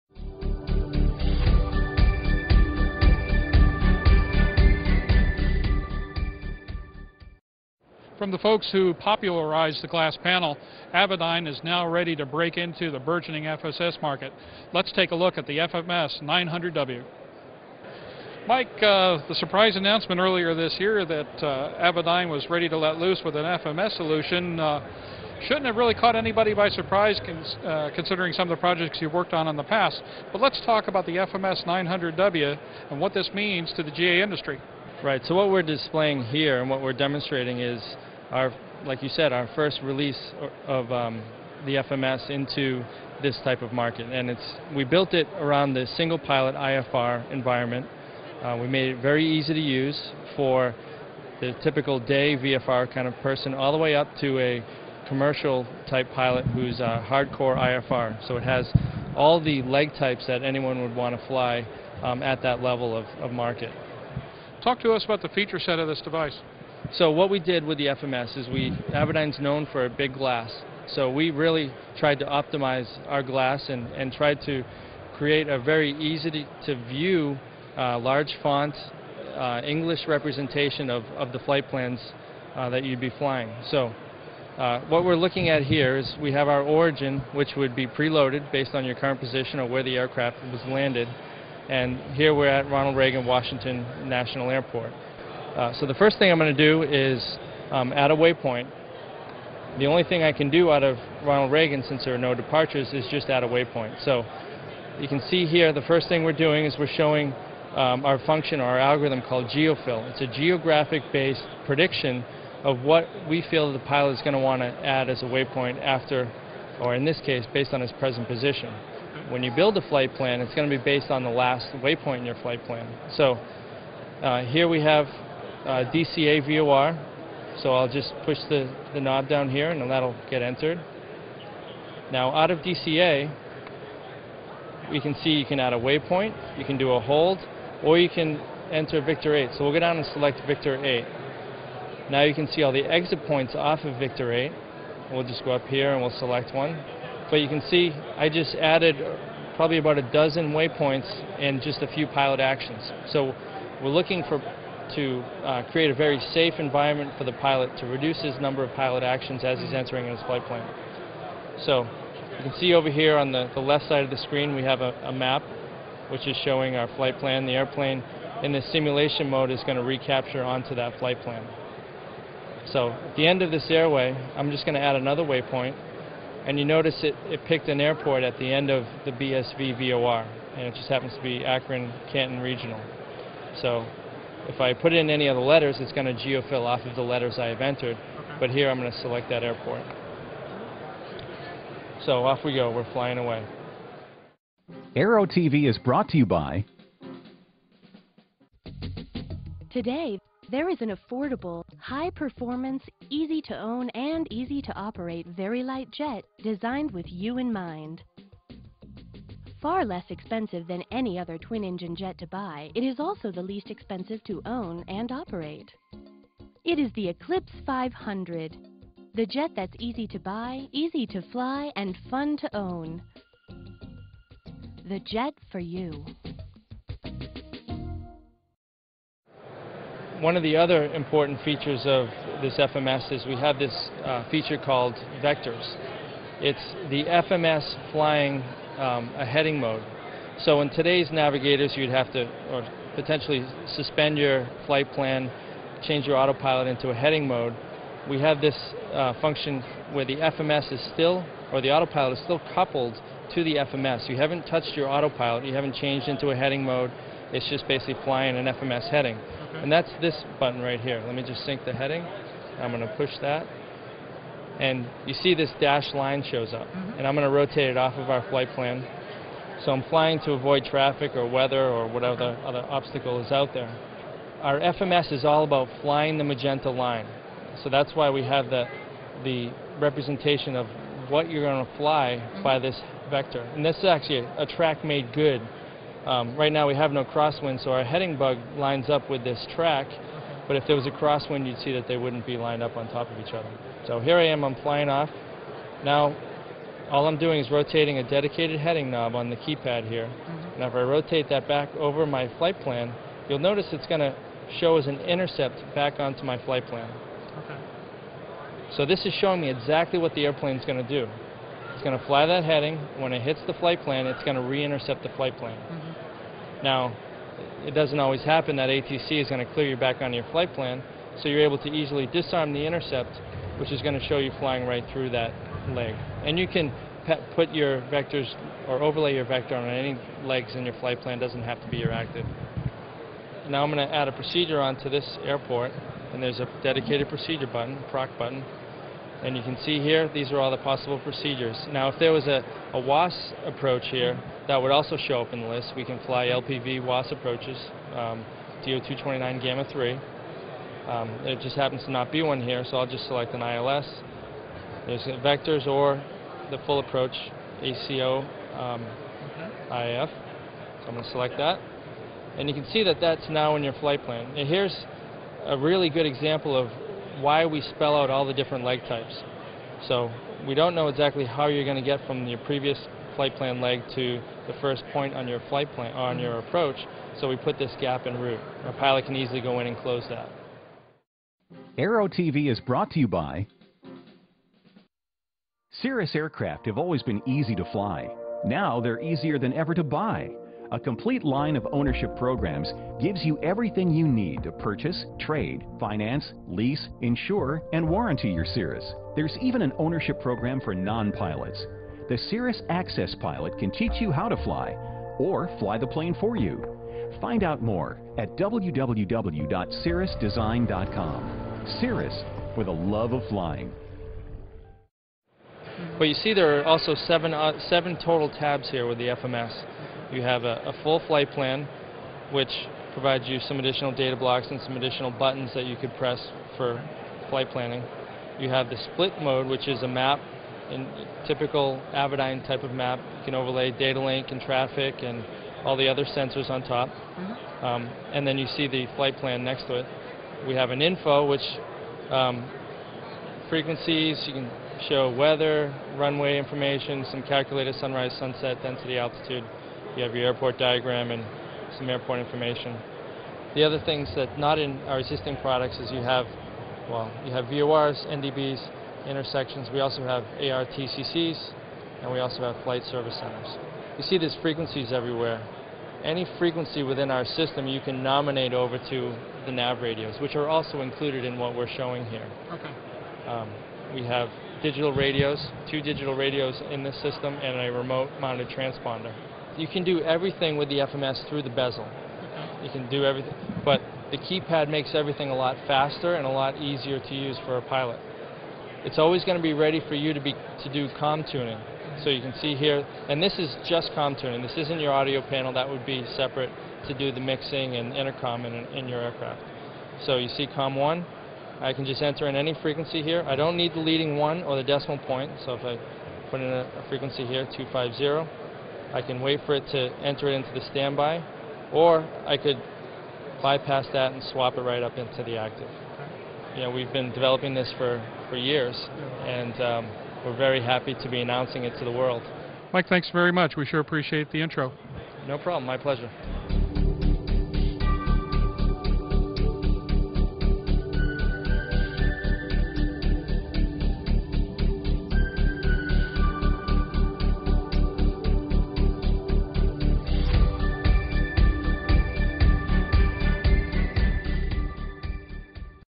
Aero-TV Gets Briefed On Avidyne's FMS900W
Entegra FMS900W Designed To Reduce Workload In Single-Pilot IFR

Avidyne recently strutted its stuff before the Aero-TV crews with a demo of their upcoming FMS900W FMS system. In the works for some time, the new Entegra FMS900W WAAS-enabled GPS/NAV/COM Flight Management System (FMS) is designed to reduce workload for single-pilot IFR operations. The FMS900W provides a fully-redundant, state-of-the art VHF and TSO C146b Gamma 3-compliant turbine-class FMS capability for all GA aircraft.

Utilizing a fully-modular architecture, the FMS900W Flight Management System is an enhancement to Avidyne's Entegra Integrated Flight Deck platform and is designed specifically to take advantage of Entegra's large-format displays and Byteflight peer-to-peer databus architecture.

FMS900W system components include single or dual remote-mount LRUs, each with Avidyne's GPS723 WAAS/RNP GPS Sensor and a DVX740 VHF NAV/COM module and optional ACD215 Control/Display Units. Avidyne's GPS723 sensor is a fully compliant RTCA DO-229D, Gamma 3 navigation system, and is designed to meet Required Navigation Performance (RNP-0.3).

Avidyne tells ANN that it is nearing completion of the product design phase and will soon be entering into the certification phase of the Entegra FMS900W program.

Aero-TV Gets A Solid Briefing On The NEW Avidyne FMS900W!